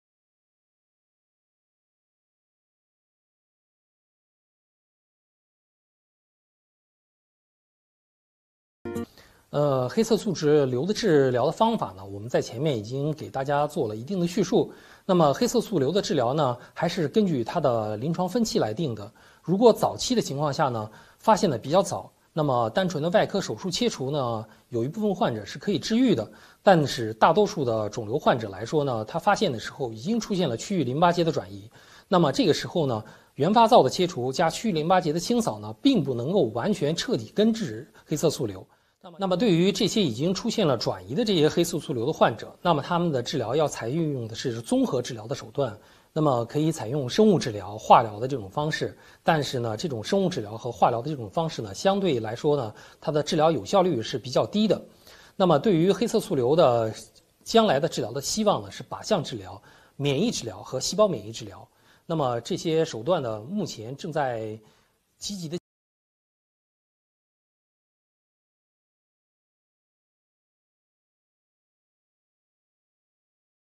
黑色素瘤的治疗方法有哪些 孔莺 西安交通大学第一附属医院
黑色素瘤的治疗的方法,在前面已经给大家做了一定的叙述,黑色素瘤的治疗根据他的临床分期来定的。如果早期的情况下,发现的比较早,单纯的外科手术切除有一部分患者是可以治愈的。但是大多数的肿瘤患者,发现的时候,已经出现了区域淋巴结的转移,这个时候原发灶的切除,加区域淋巴结的清扫,并不能够完全彻底根治黑色素瘤。对于这些已经出现了转移的这些黑色素瘤的患者,他们的治疗要采用的综合治疗的手段。

What are the treatment methods of melanoma
The treatment of melanoma, I have already given you some narration, The treatment of melanoma is based on his clinical stage. In the early case, It was found earlier, Some patients can be cured by simple surgical resection. But most cancer patients, At the time of discovery, There has been regional lymph node metastasis, At this time, the primary focus was removed, Plus regional lymph node dissection, It can not completely cure melanoma. For these melanoma patients who have already developed metastasis, Their treatment should adopt the comprehensive treatment method.